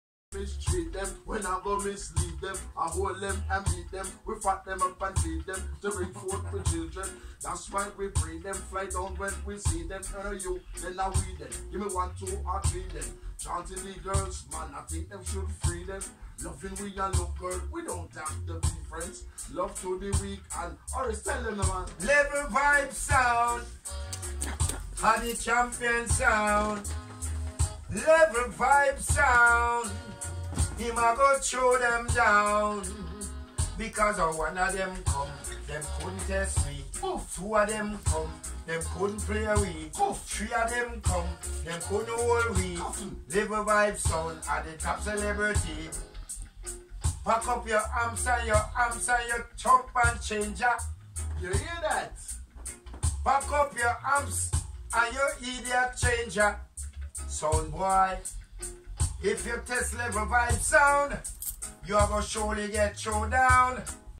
Horace Martin and King madix dubplate. Scion success.
Dubplate session Horace Martin king Madix Scion success proud to be studio LONDON UK.